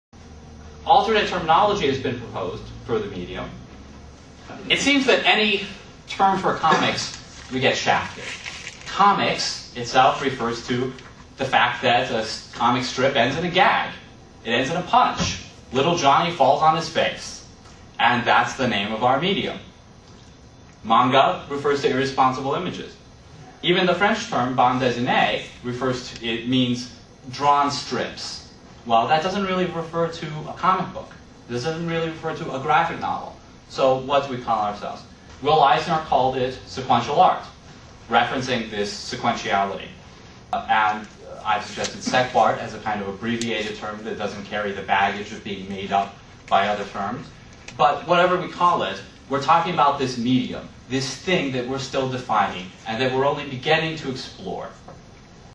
Why Comics Matter #1.5: Terminology
A lecture by Dr. Julian Darius, delivered 5 January 2006 at Glen Carbon Centennial Library (Glen Carbon, Illinois).